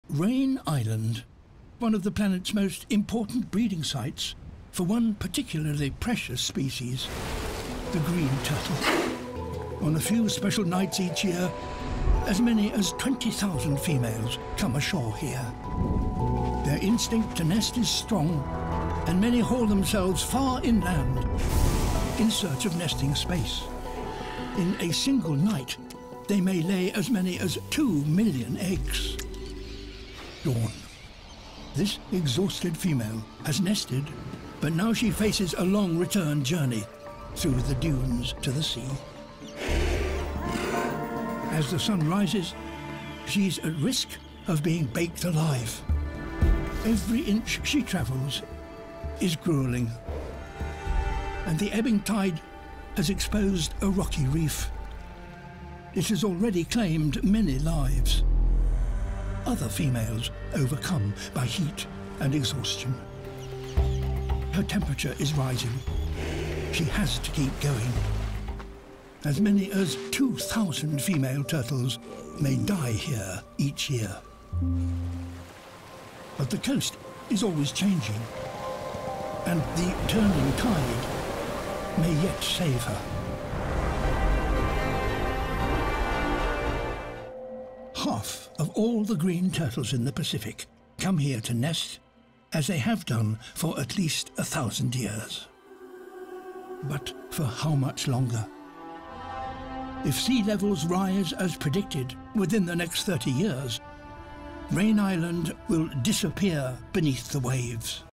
Raine Island: The World’s Largest Green Turtle Nesting Site
Deep in Australia’s Great Barrier Reef lies Raine Island, the world’s most important green turtle nesting site. Every year, tens of thousands of female green turtles return to this small island to lay their eggs—a ritual that has continued for millions of years.

In this video, you’ll discover:
🐢 Why Raine Island is the largest green turtle rookery on Earth
🌊 The incredible journey of female turtles returning to nest
⚠️ The challenges turtle hatchlings face from predators and climate change

Join us as we uncover the story of survival, resilience, and one of the greatest wildlife spectacles on our planet.